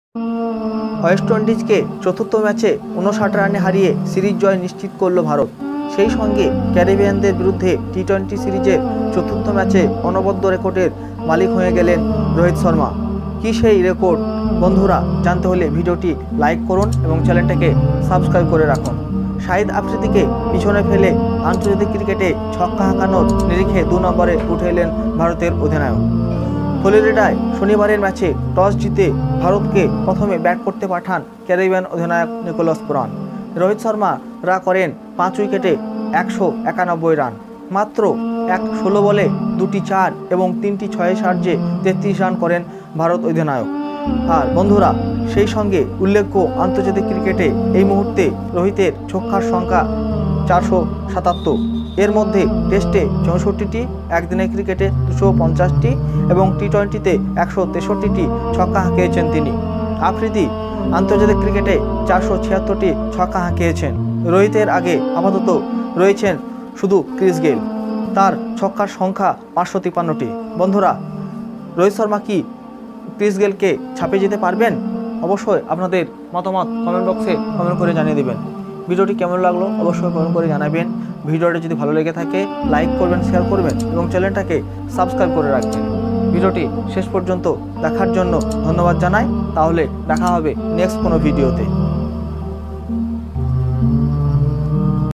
India vs West Indies:চতুর্থ ম্যাচেই ভারতের পকেটে সিরিজ, ছক্কা হাঁকিয়ে রেকর্ড রোহিতের
India vs WestIndies 4th T20i: Rohit Sharma Breaks Shahid Afridi's BIG Record

CRICKET IN BENGALI

KD Tv

UTV News24

CRICKET LOVERS

cricket lovers

Cricket Lovers